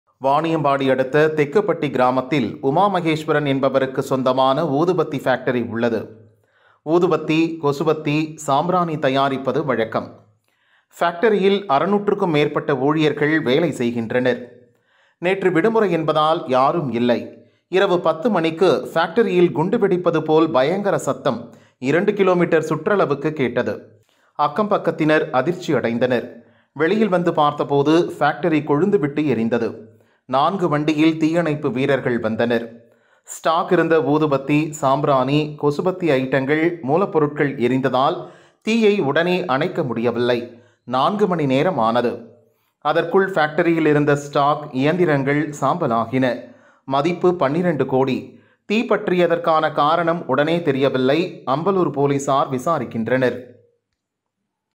ஊதுவத்தி பேக்டரியில் குண்டு சத்தம் 12 கோடி பொருட்கள் தீயில் சாம்பல்
ஊதுவத்தி பேக்டரியில் குண்டு சத்தம் 12 கோடி பொருட்கள் தீயில் சாம்பல் | Fire Accident | Vaniyambadi | Dinamalar#Fire